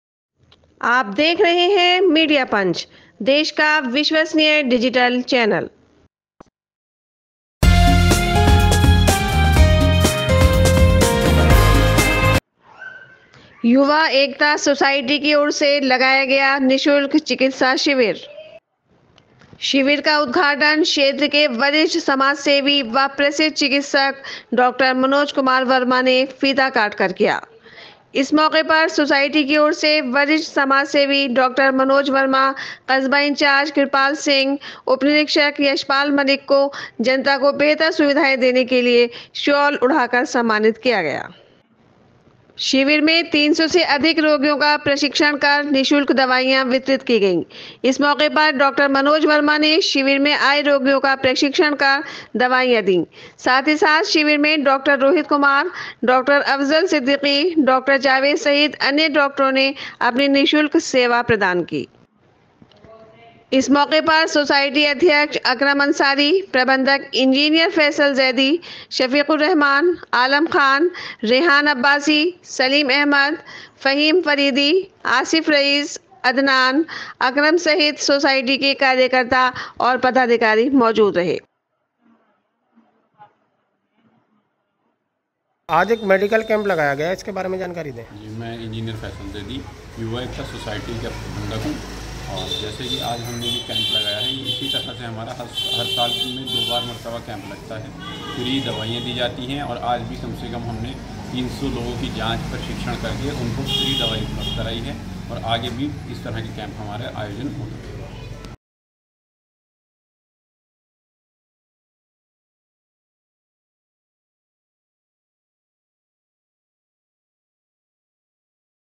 युवा एकता सोसायटी की और से लगाया गया निशुल्क चिकित्सा शिविर
शिविर का उद्घाटन क्षेत्र के वरिष्ठ समाजसेवी व प्रसिद्ध चिकित्सक डॉ मनोज कुमार वर्मा ने फीता काटकर किया।
इस मौके पर सोसायटी की और से वरिष्ठ समाजसेवी डॉ मनोज वर्मा,कस्बा इंचार्ज किरपाल सिंह ,उपनिरीक्षक यशपाल मलिक को जनता को बेहतर सुविधाएं देने के लिए शॉल ओढ़ाकर सम्मानित किया।
शिविर में 300 से अधिक  रोगियों का प्रशिक्षण कर निशुल्क दवाइयां वितरित की गई इस मौक़े पर डॉ मनोज वर्मा शिविर में आए रोगियों का प्रशिक्षण कर दवाइयां दी साथ ही साथ शिविर में डॉ रोहित कुमार, डॉ अफज़ल सिद्दीक़ी,डॉ जावेद सहित अन्य डॉक्टरों ने अपनी निशुल्क सेवा प्रदान की।
इस मौके पर सोसायटी अध्यक्ष अकरम अंसारी,प्रबन्धक इंजी फैसल ज़ैदी,शफीकुर्रहमान, आलम ख़ान, रेहान अब्बासी,सलीम अहमद,फ़हीम फरीदी,आसिफ़ रईस अदनान अकरम सहित सोसायटी के कार्यकर्ता और पदाधिकारी मौजूद रहे।
बाईट-इंजी फैसल ज़ैदी प्रबन्धक युवा एकता सोसायटीbijnordastak